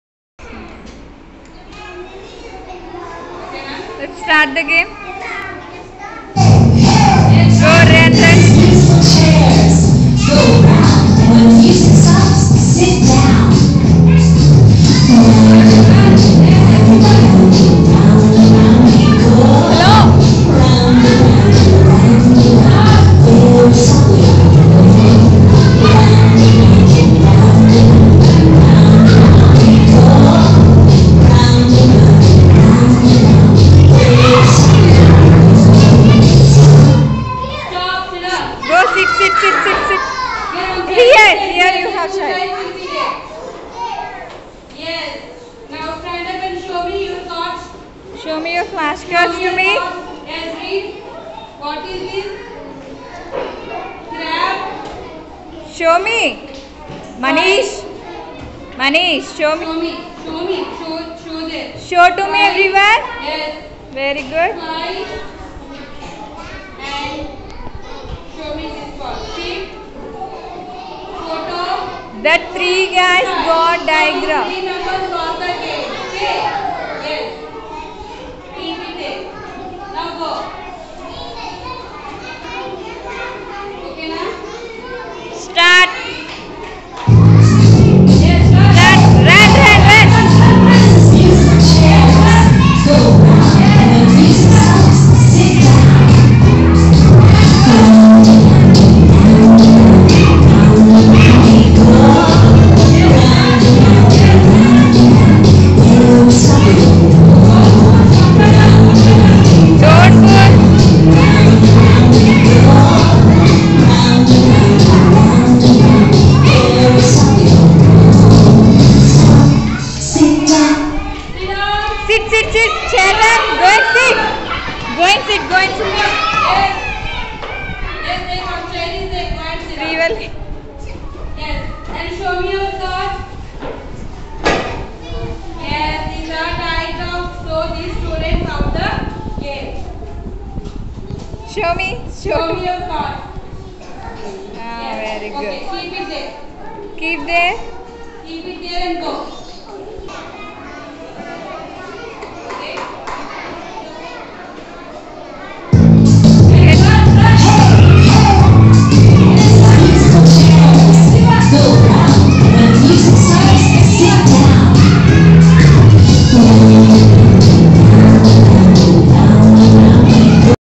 Thrilling and easy learning musical chair game of my kids
Thrilling and learning musical chair game of my dear kids